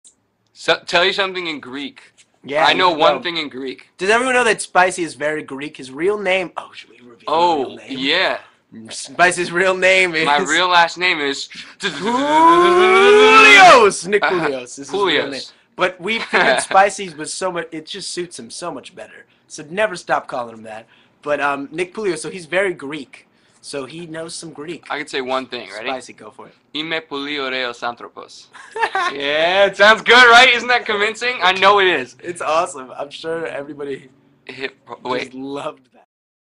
Nick Spicy Brown (Nicholas Poulios) Speaks Greek
Scene from the chat made on 22 December 2012 with Max Schneider (@Maxgschneider) and Nicholas Poulios (@Nickspicybrown).
I asked Nick to say something in Greek and Nick did it!!!